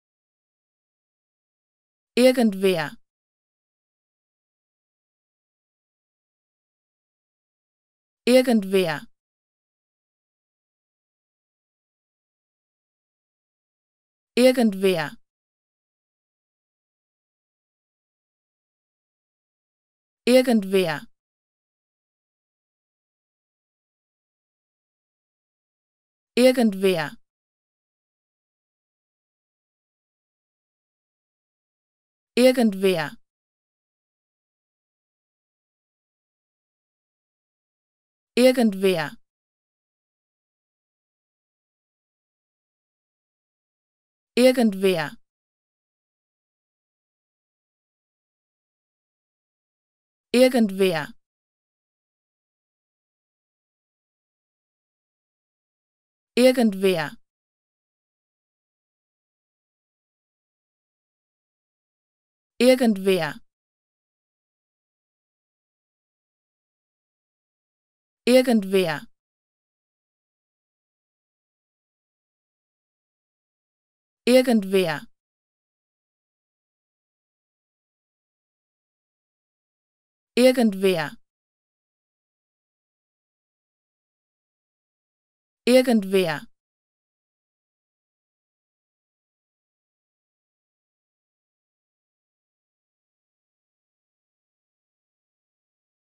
How to Pronounce ''irgendwer'' (someone) Correctly in German
Learn how to say ''irgendwer'' in German with this short pronunciation tutorial.